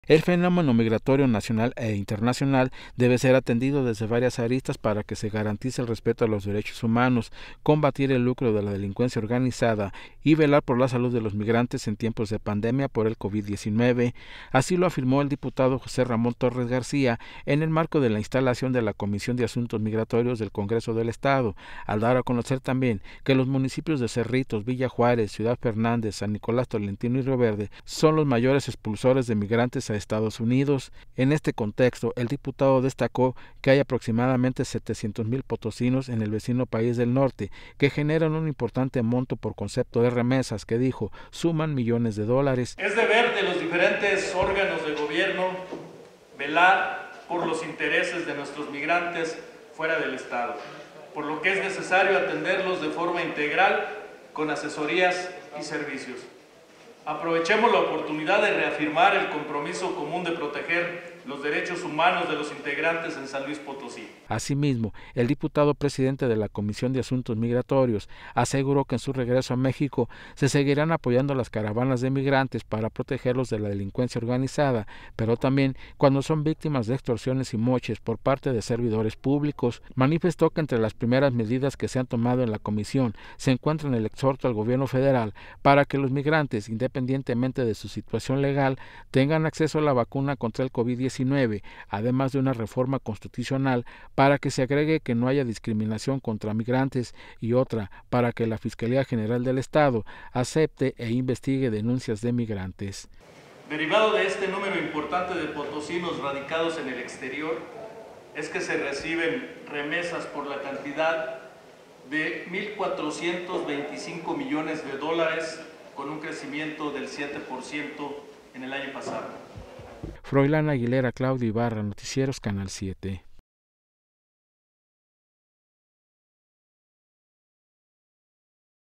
Cerca de 7 mil potosinos generan remesas por más de 1 millón 400 mil dólares: Torres García.
Cerca de 7 mil potosinos, que viven en EEUU, generan remesas por más de 1 millón 400 mil dólares: Torres García.
• Instalan Comisión de Asuntos Migratorios.

SLP.- El fenómeno migratorio nacional e internacional debe ser atendido desde varias aristas para que se garantice el respeto a los derechos humanos, combatir el lucro de la delincuencia organizada, y velar por la salud de los migrantes en tiempos de pandemia por el COVID-19.

Así lo afirmó el diputado José Ramón Torres García en el marco de la instalación de la Comisión de Asuntos Migratorios del Congreso del Estado, al dar a conocer también que los municipios de Cerritos, Villa Juárez, Ciudad Fernández, San Nicolás Tolentino y Rioverde, son los mayores expulsores de migrantes a Estados Unidos.

En este contexto, el diputado destacó que hay unos 700 mil potosinos en el vecino país del norte que generan una importante monto por concepto de remesas que dijo, suman millones de dólares.

Asimismo, el diputado presidente de la Comisión de Asuntos Migratorios aseguró que en su regreso a México se seguirán apoyando las caravanas de migrantes, para protegerlos de la delincuencia organizada pero también cuando son víctimas de extorsiones y moches, por parte de servidores públicos.

Manifestó que entre las primeras medidas que se han tomado en la Comisión se encuentran el exhorto al Gobierno Federal, para que los migrantes, independientemente de su situación legal, tengan acceso a la vacuna contra el COVID-19, además de una reforma constitucional para que no haya discriminación contra migrantes, y otra para que la Fiscalía General del Estado acepte e investigue denuncias de migrantes.